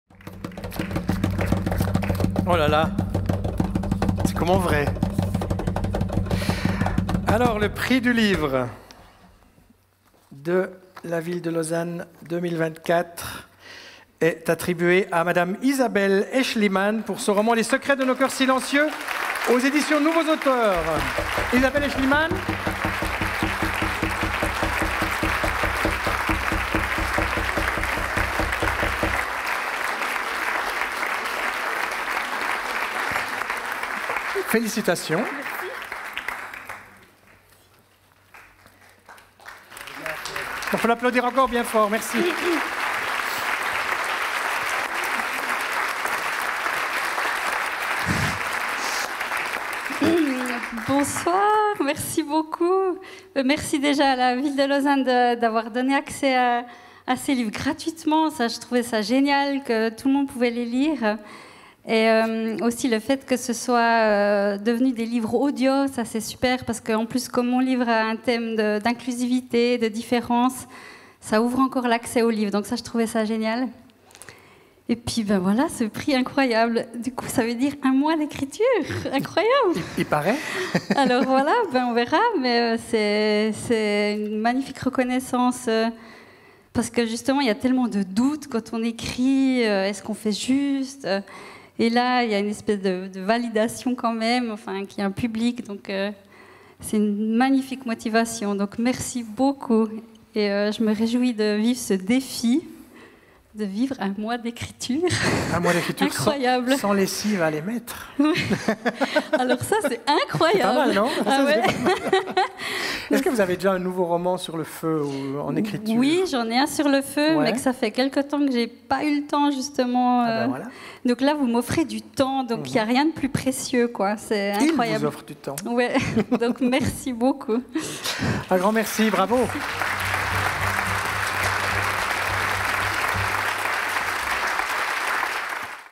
Soirée de remise du Prix du livre de la Ville de Lausanne 2024
Emotions, musique, lectures et discussions au menu de la cérémonie de remise du Prix du livre de la Ville de Lausanne 2024 lundi 4 mars au Théâtre de Vidy-Lausanne! Revivez en accéléré la cérémonie: la violoncelliste Sara Oswald, la comédienne Shin Iglesias et le comédien Michel Sauser, les comédiennes et comédiens de l’atelier-théâtre de l’Espace Mont-Blanc, ainsi que les cinq autrices et auteurs de la Sélection. Le tout sous la houlette de notre maître de cérémonie, Lionel Frésard. Au final, une lauréate: Isabelle Aeschlimann pour «Les secrets de nos cœurs silencieux» (éditions Les Nouveaux Auteurs).